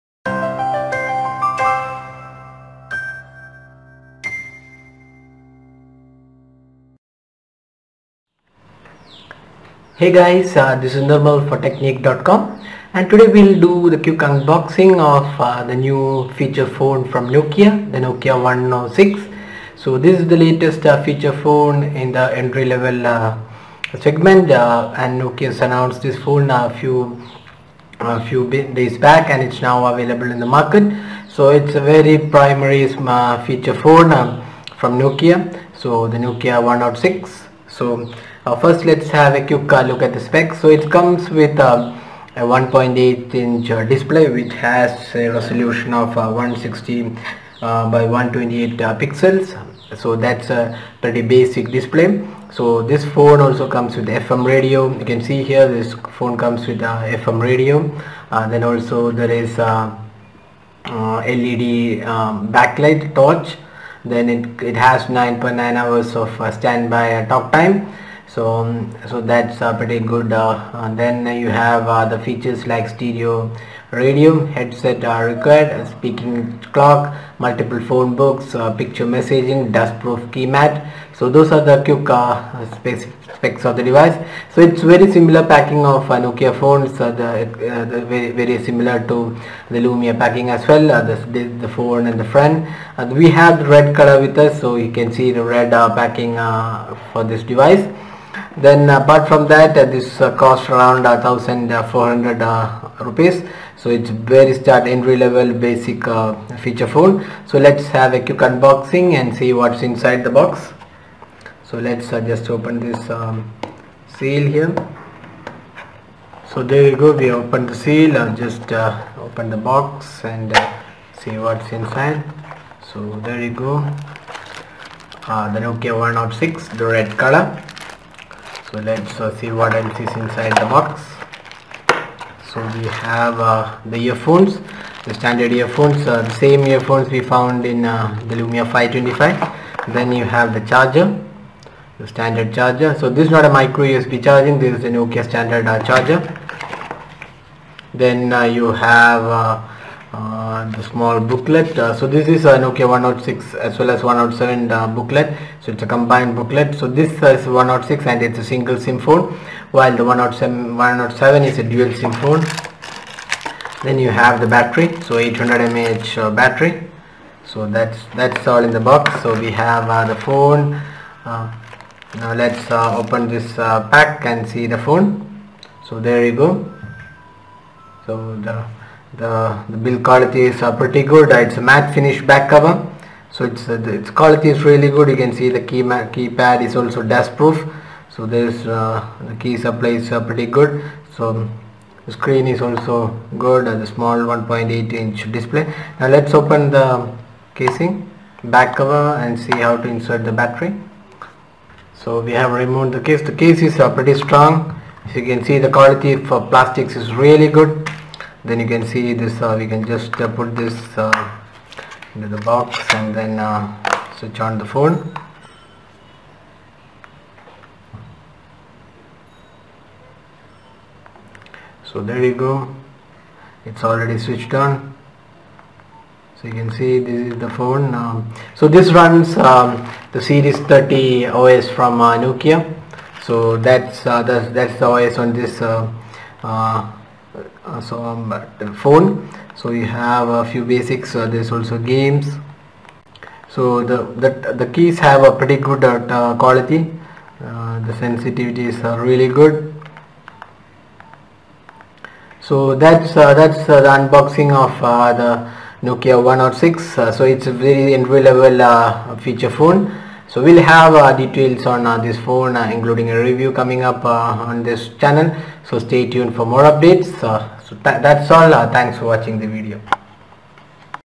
Nokia 106 Unboxing
Unboxing video of feature phone from Nokia, the Nokia 106 with 1.8 inch display, FM Radio and torch light.